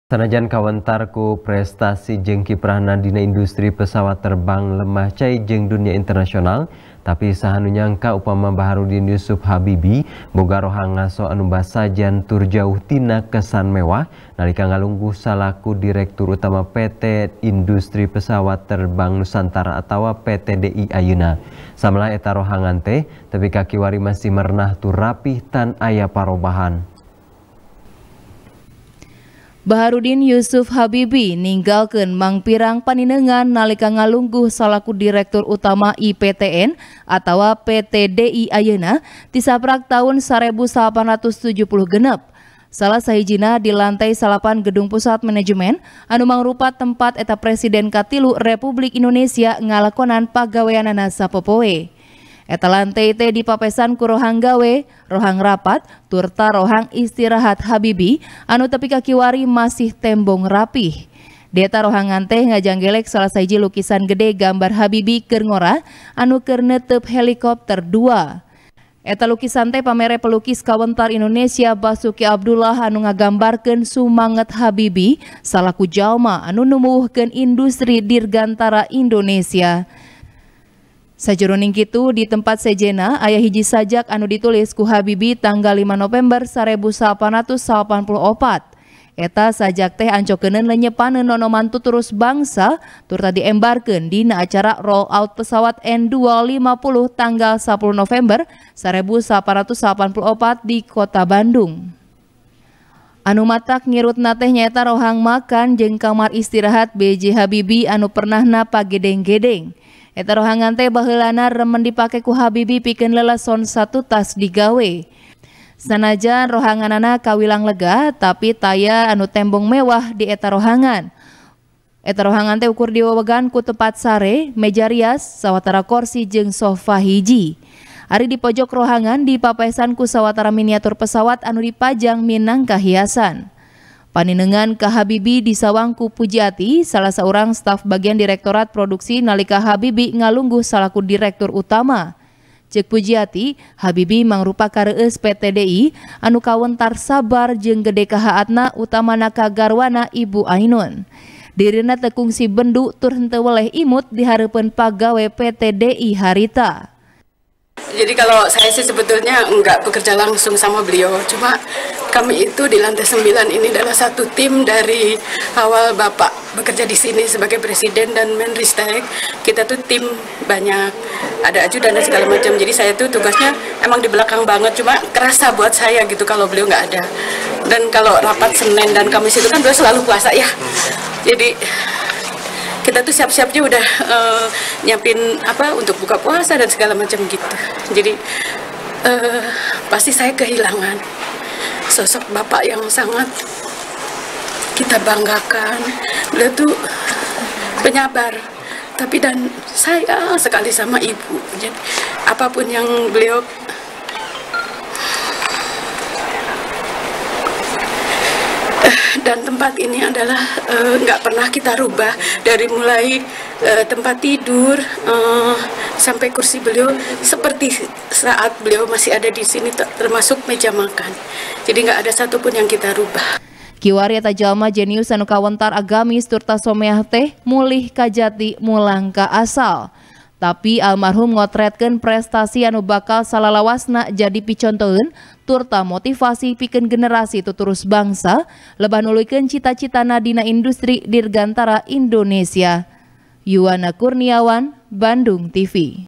Mitineung Kabasajanan B.J. Habibie Nalika Ngalungguh Dirut IPTN | TP | BANDUNGTV
TP | BANDUNGTV | IWAN M. RIDWAN | Sanajan Kawentar Ku Prestasi Jeung Kiprahna Dina Industri Pesawat Terbang Lemah Cai Jeung Dunya, Tapi, Saha Nu Nyangka Upama Baharudin Jusuf Habibie Boga Rohang Ngaso Anu Basajan Tur Jauh Tina Kesan Mewah Nalika Ngalungguh Salaku Direktur Utama PT Industri Pesawat Terbang Nusantara Atawa PT DI Ayeuna. Samalah Eta Rohang Teh, Tepi Ka Kiwari Masih Merenah Tur Rapih Tan Aya Parobahan.